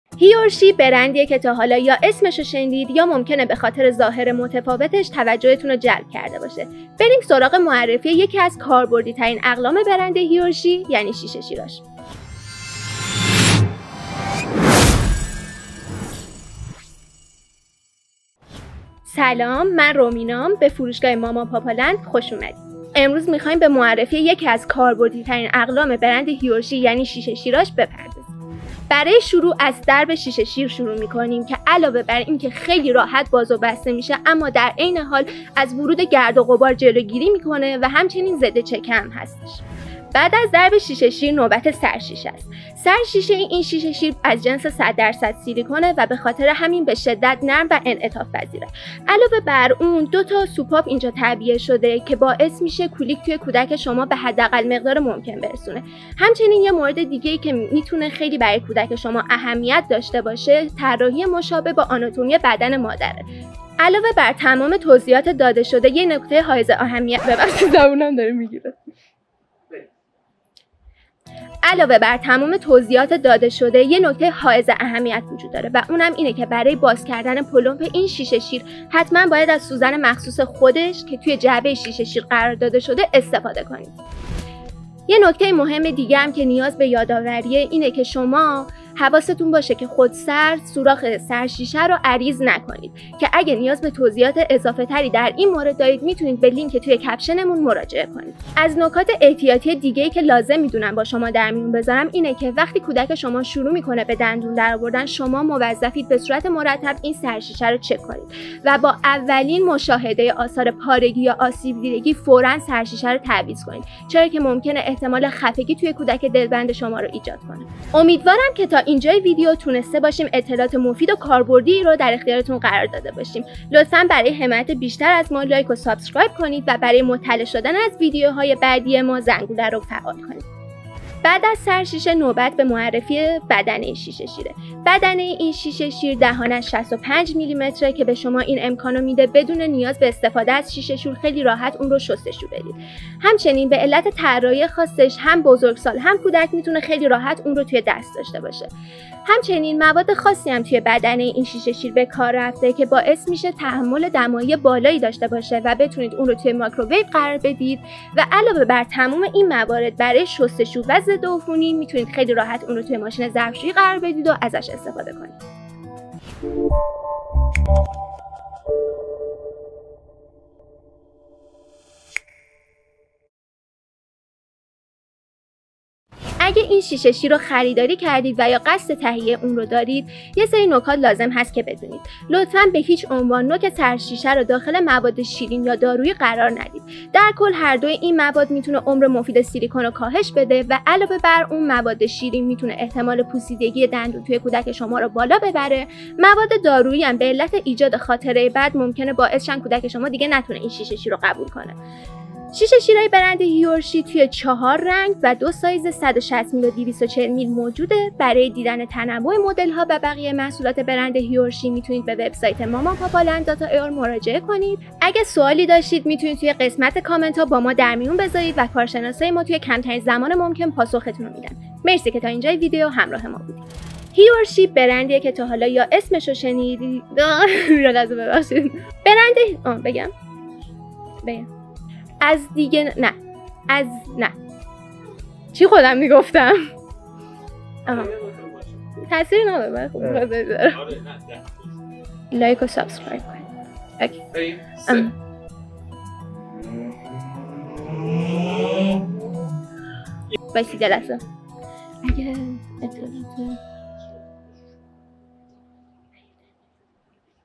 معرفی و بررسی شیشه شیر هی اور شی | آیا ارزش قیمتشو داره؟
برند هی اور شی (heorshe) یکی از معروف ترین برند های شیه شیر در دنیا هست، در این ویدیو همه جزئیات این شیشه شیر را بررسی میکنیم و مهمترین دلایل معروفیت آن را بیان میکنیم.

در این ویدیو با ماماپاپالند همراه باشید تا یکی دیگه از مهمترین قسمت های سیسمونی یعنی شیشه شیر بپردازیم.

با آیدی mamapapaland ما را در شبکه های اجتماعی دنبال کنید، همچنین از وب سایت ما دیدن کنید.